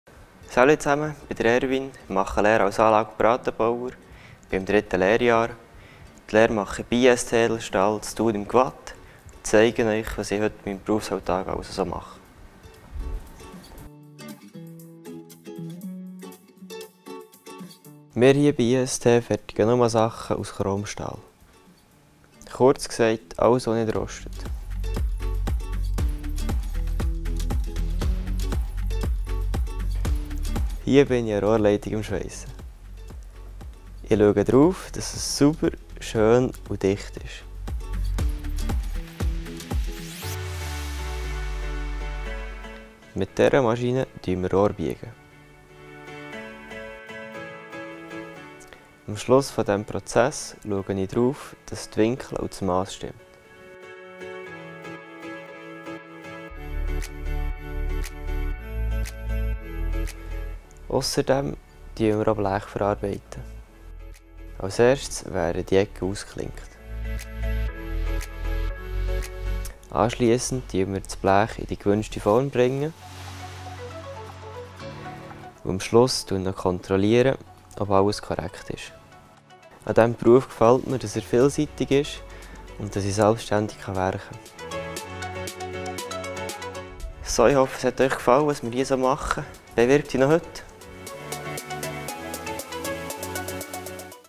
Lehre als Anlagen- und Apparatebauer/in EFZ bei IST-Edelstahl-Anlagenbau AG
Ein Einblick in den Alltag der Lehre als Anlagen- und Apparatebauer/in EFZ bei IST-Edelstahl-Anlage